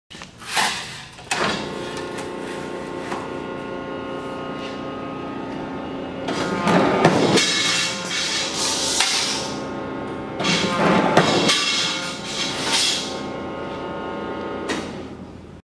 Used Nelson 52" x 16 Ga. Hydraulic Shear For Cutting Sheet Metal - now offered for sale SOLD
Used Nelson 52" x 16 Ga. Hydraulic Shear For Cutting Sheet Metal - now offered for sale.

Condition - Used, good
Two Front Sheet Supports
Manual Back Gauge
Foot Pedal Operated

WE BUY USED EQUIPMENT!!

 Item # 1659